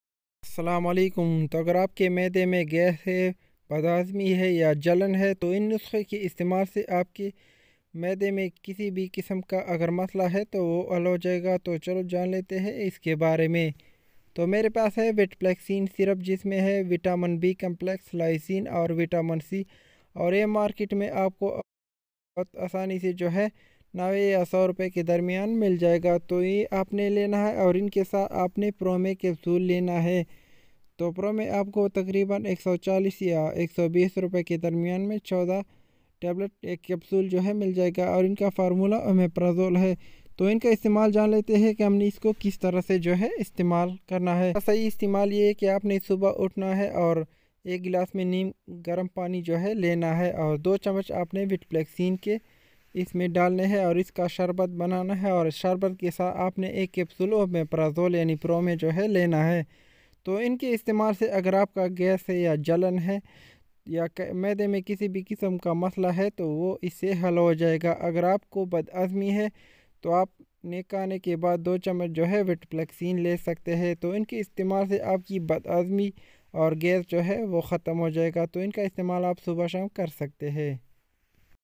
Gass Badhazmi Jalan Heart Bornig Acidity ka ilaj Urdu Hindi|VitPlexin Syp Promy cap ka istamal Urdu
hartbornig Gass Badhazmi acidity Jalan ka ilaj Urdu Hindi very simple treatment of stomach disease.

vitplexin sup uses in Urdu

Promy Cap uses in Urdu Hindi

subo sham is ko istamal karskthy ho. Khana se pehly.

Badhazmi acidity ka khatma.